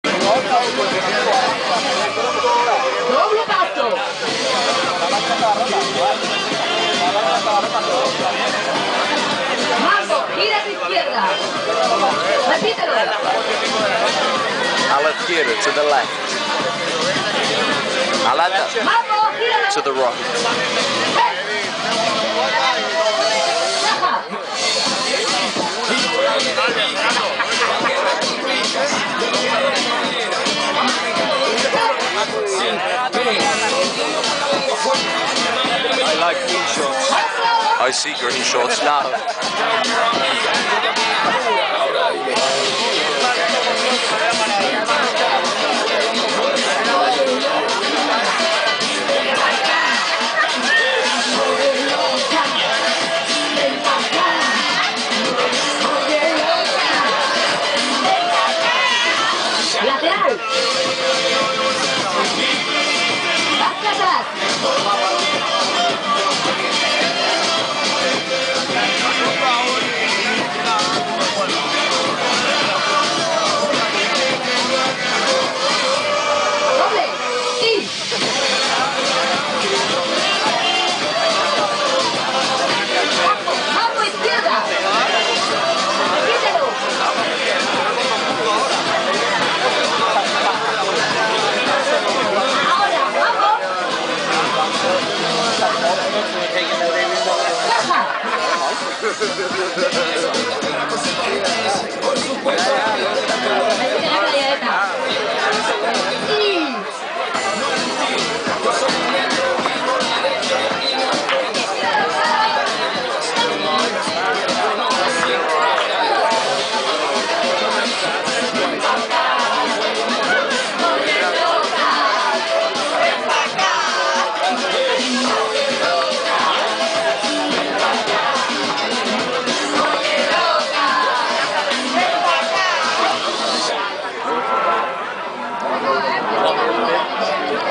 Catalonia pineda de mar Barcelona latin dancing girls
Spanish salsa dancing at moloko beach bar in  pineda de mar, Catalonia, Spain.